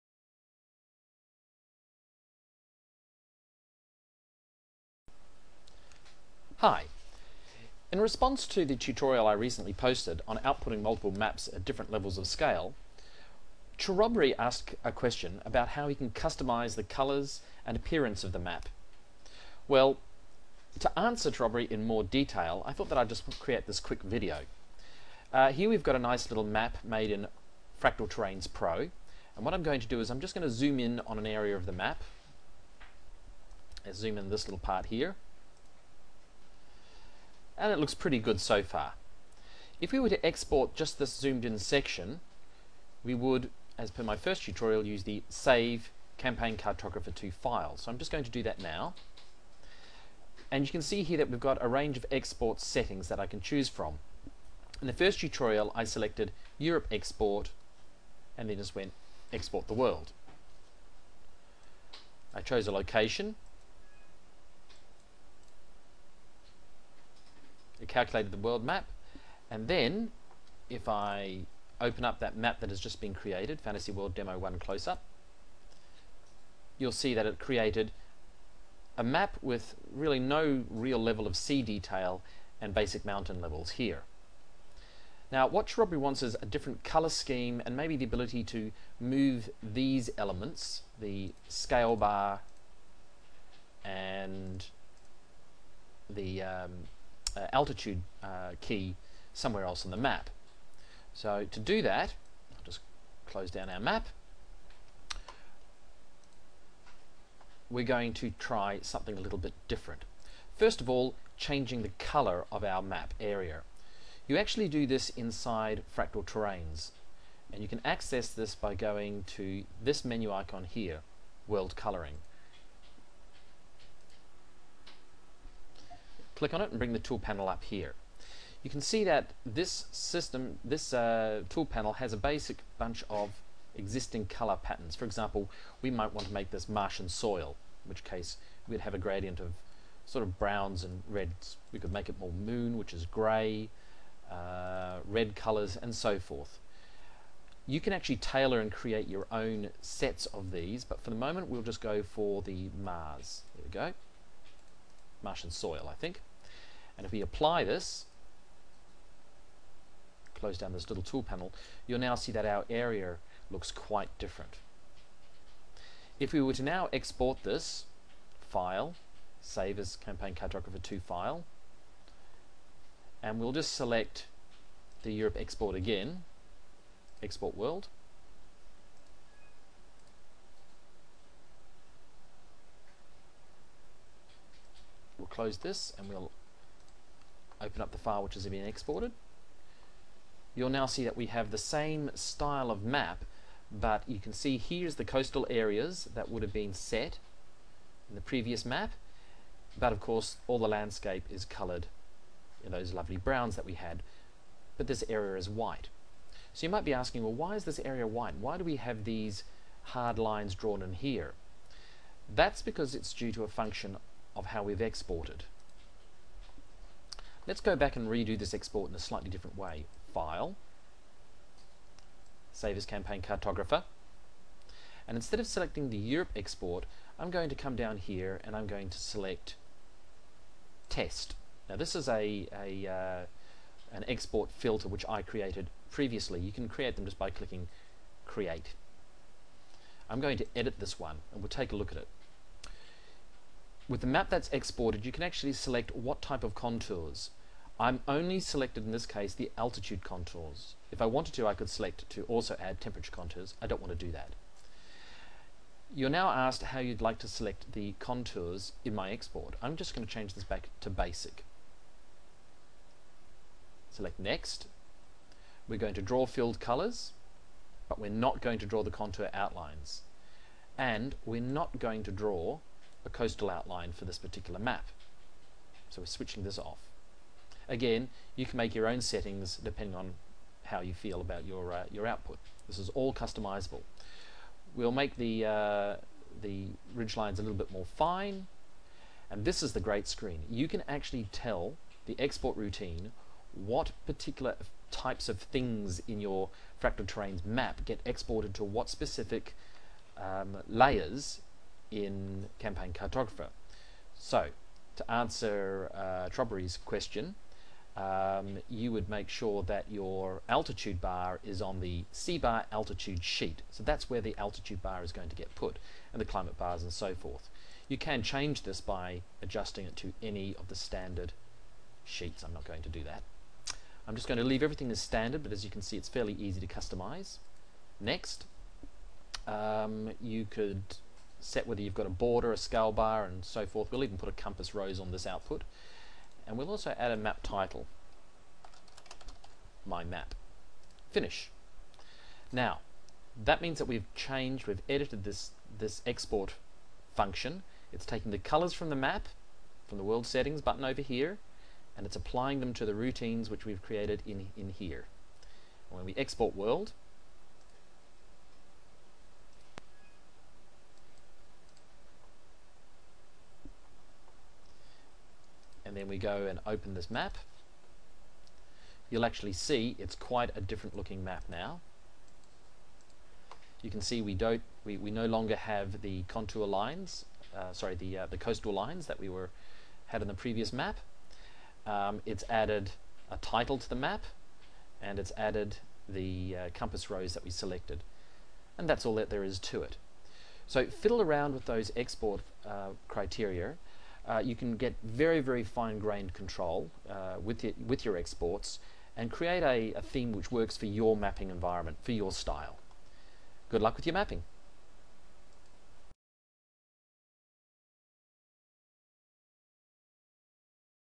FT Tutorial 3 - Customizing Exported Maps
In response to a question from one of our viewers, we delve into ways the you can customize how maps are exported from Fractal Terrains Pro.  Create you own color schemes, set how much detail should be exported, what CC3 layers are used, and much, much more.

For all of my mapping tutorials and bonus downloads, please visit my FREE master mapping program